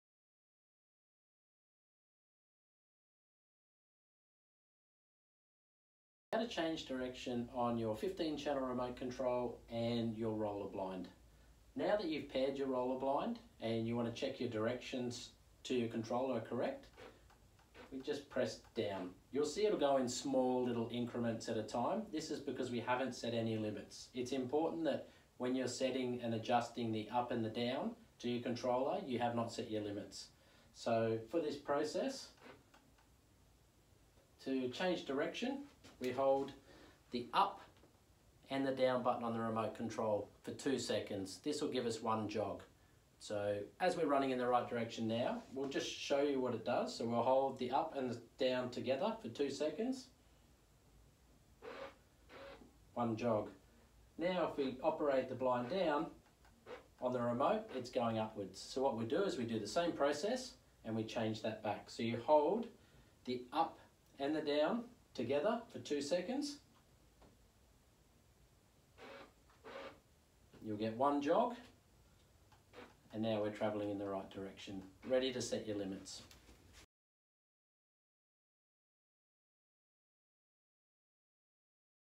WINDOWARE MOTORISATION | ROLLER BLIND MOTORS | How to change the direction of your motor
Watch one of our experts demonstrate how to change the direction of your Windoware Roller blind motor. This video applies to Windoware 25mm & 35mm DC Battery motors and 35mm A/C Wifi motors.